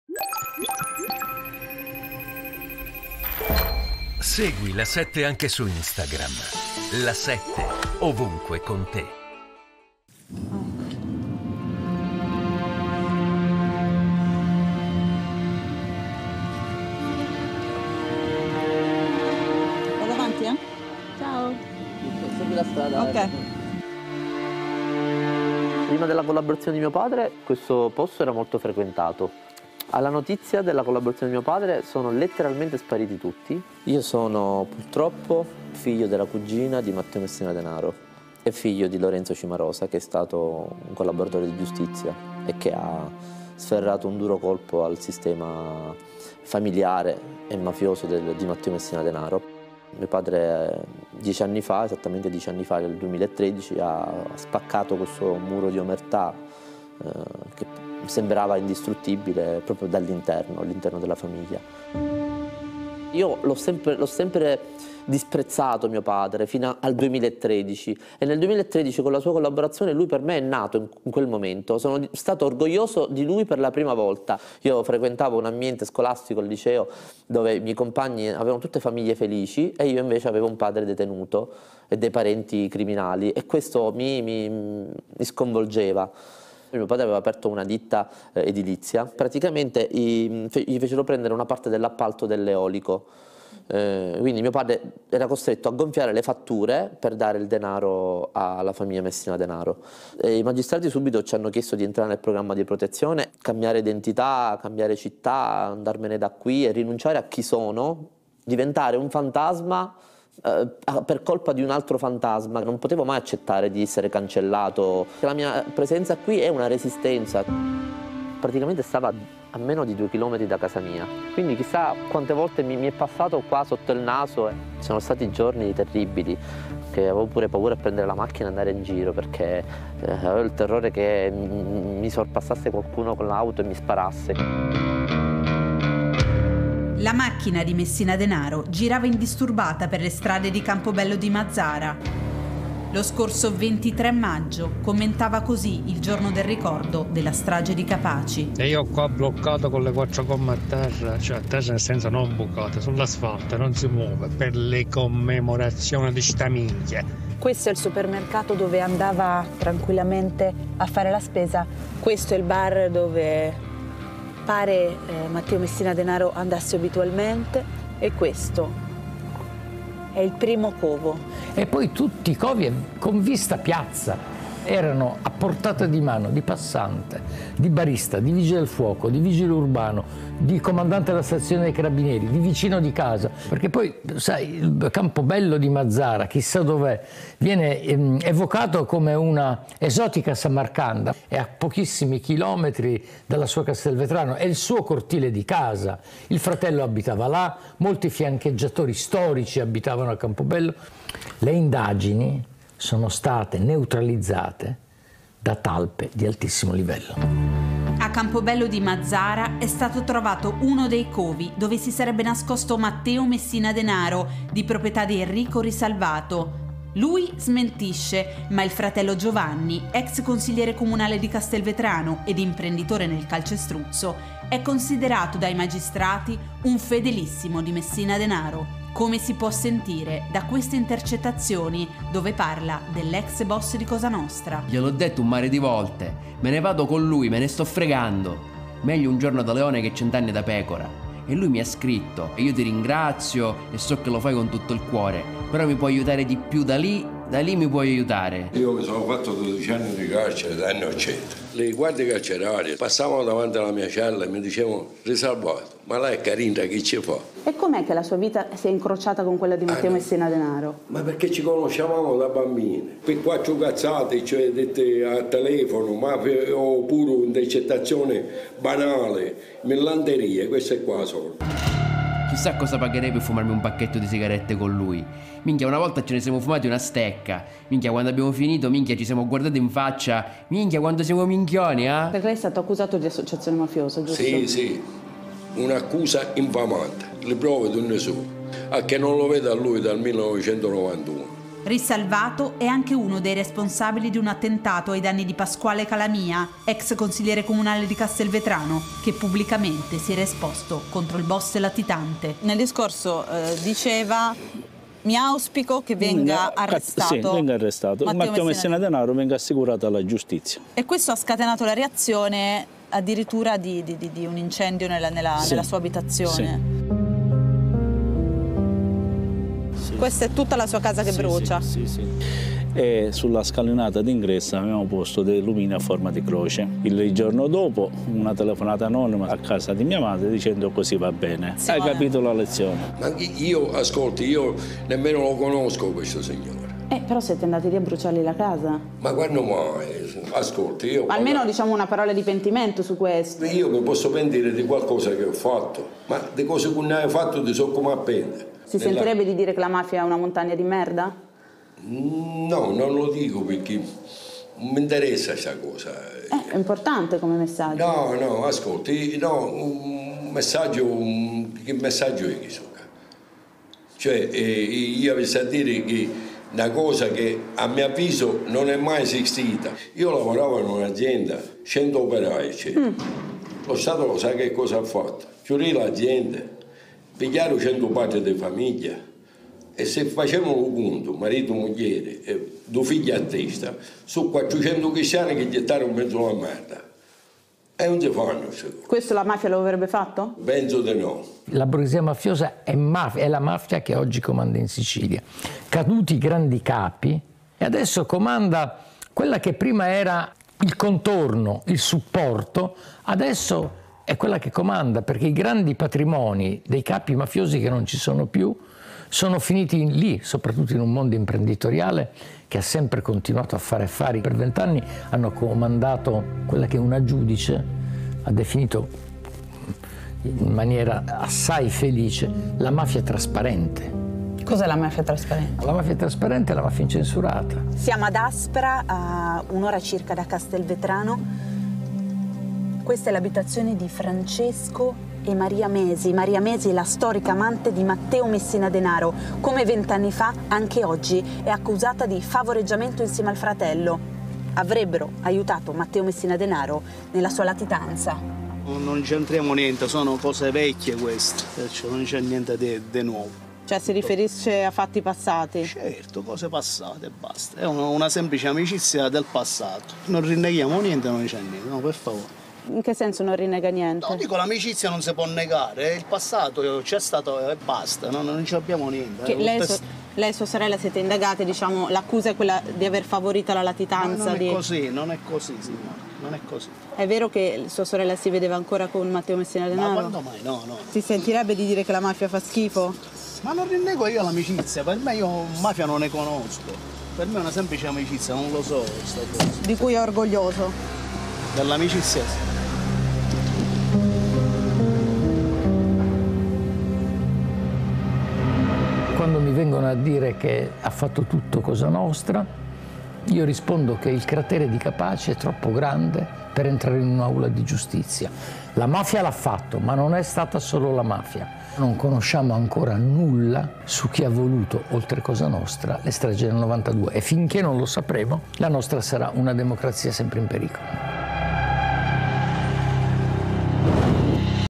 Matteo Messina Denaro: chi ha aiutato il boss?
Sara Giudice è tornata in Sicilia per ripercorrere la rete di protezione che avrebbe aiutato il boss Matteo Messina Denaro a nascondersi nel corso della sua latitanza; e ha intervistato anche Attilio Bolzoni, giornalista de “Domani”.